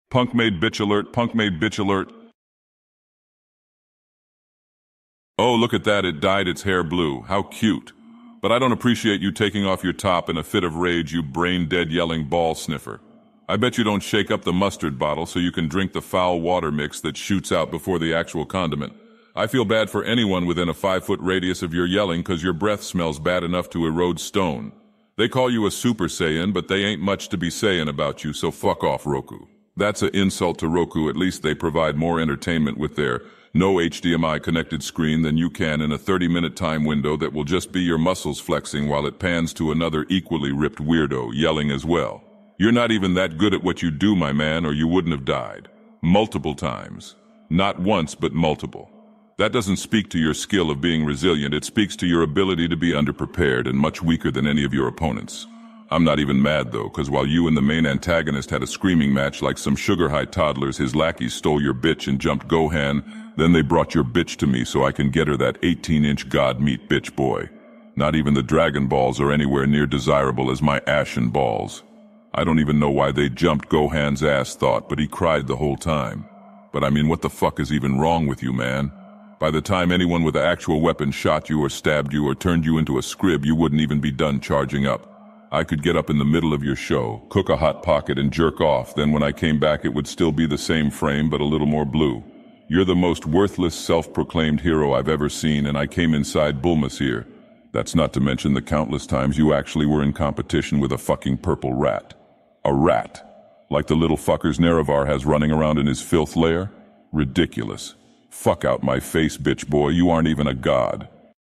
Dagoth Ur Roast Goku (Ai Voice Meme)
I wrote the script and edited the images/video. the voice is ai generated. hope you enjoy ❤ also we have a discord